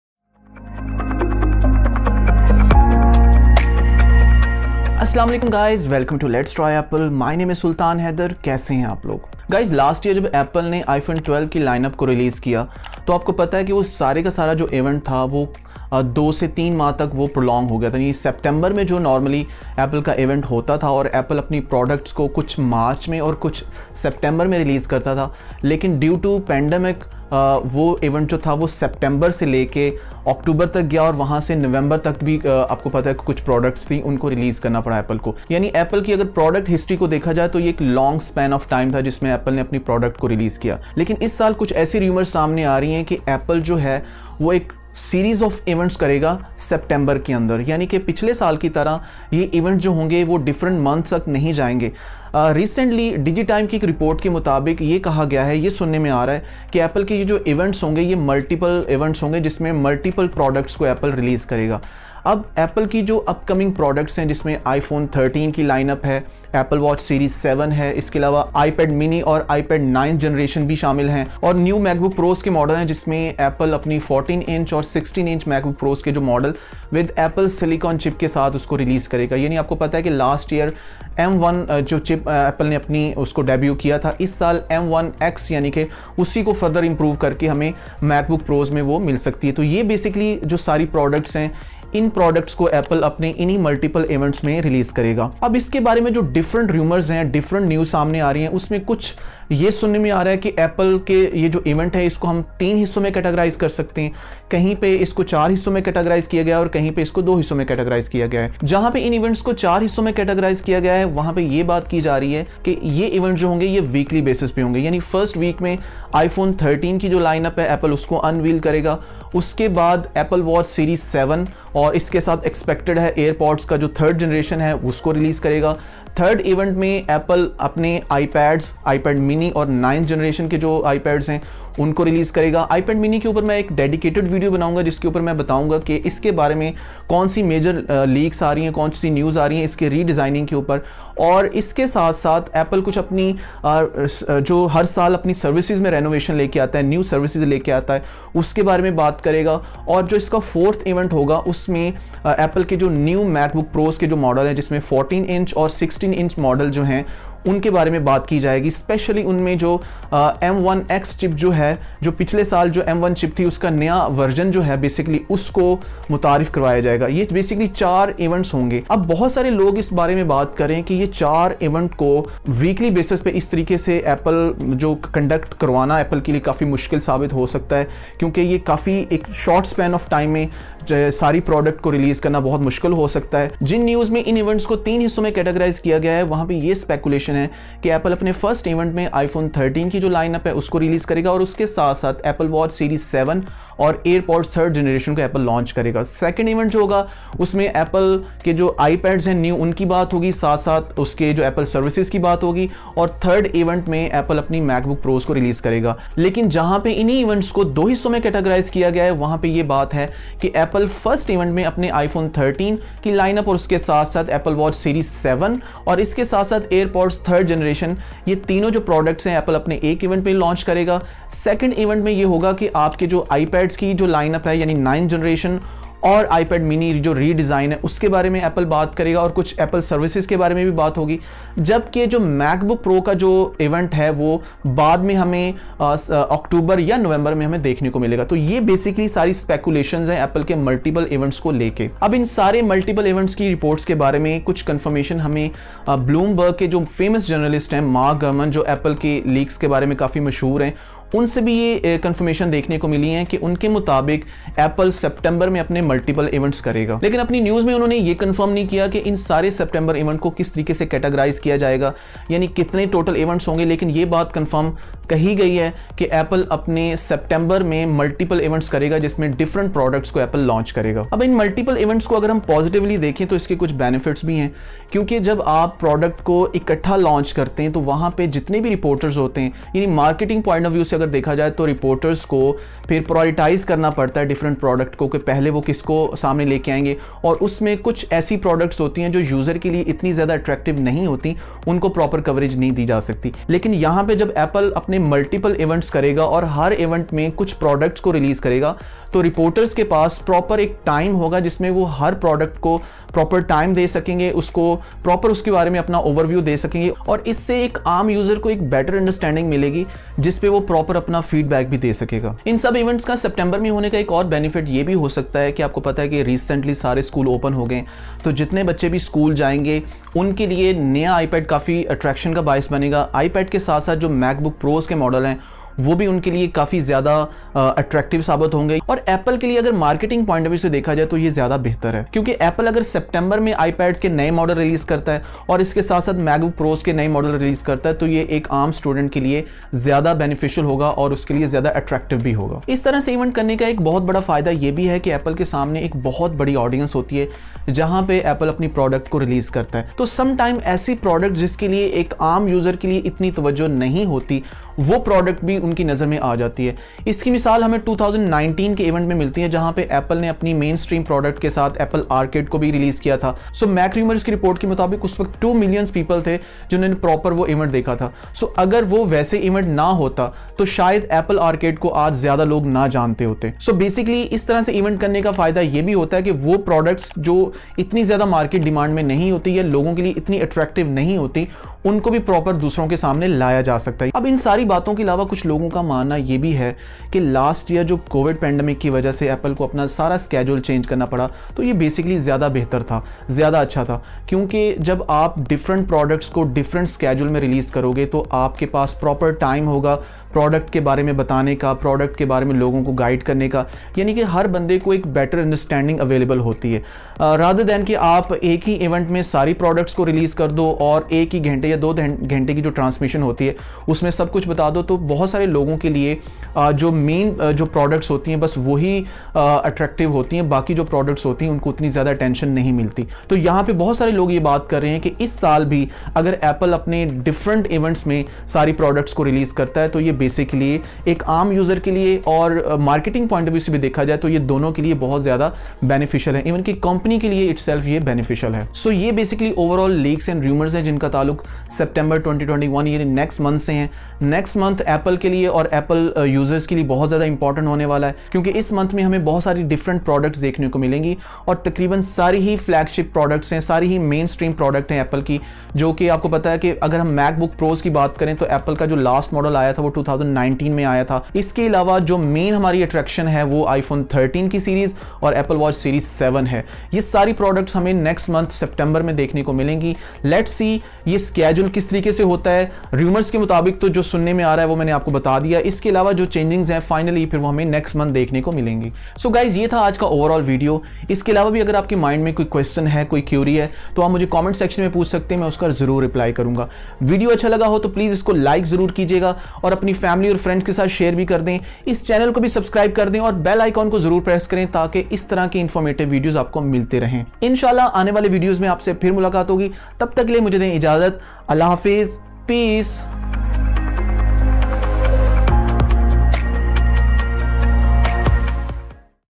Apple Major September Events | iPhone 13 | Apple Watch Series 7 | Let's Try Apple
Every year Apple September events mean new flagship products specially iPhone this year (iPhone 13 models) Apple watches this year (Apple Watch Series 7) so next month is also going to happen the same big thing. So for further detail please watch the whole video.👍

iPhone 13 | Apple Watch Series 7 | iPad mini Redesigned 2021 | Apple Major September Events | Let's Try Apple